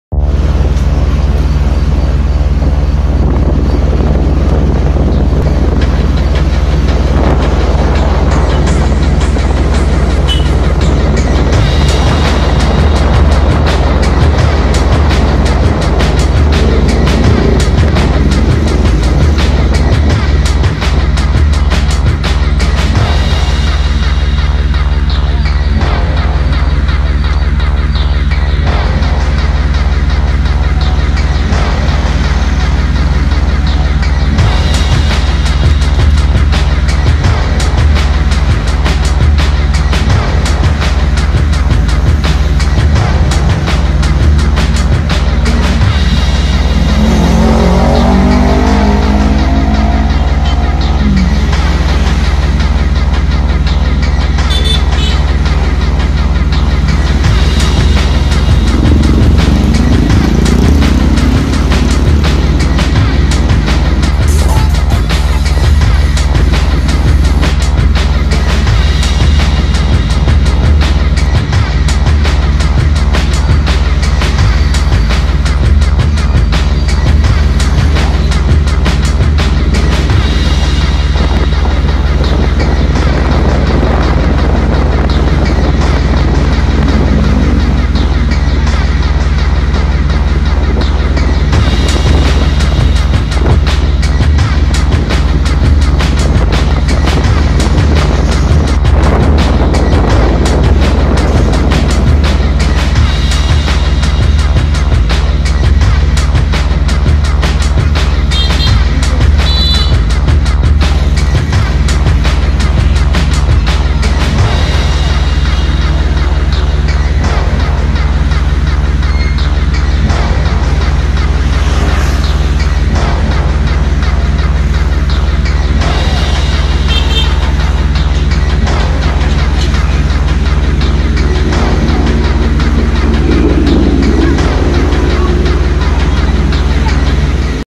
Lihat Ogoh-ogoh Sebelum Hari Raya Nyepi di Denpasar
Berkeliling di Kota Denpasar untuk melihat pameran Ogoh-ogoh (boneka raksasa). Hari Raya Nyepi jatuh pada tanggal 7 Maret 2019 menyambut tahun baru Saka 1941.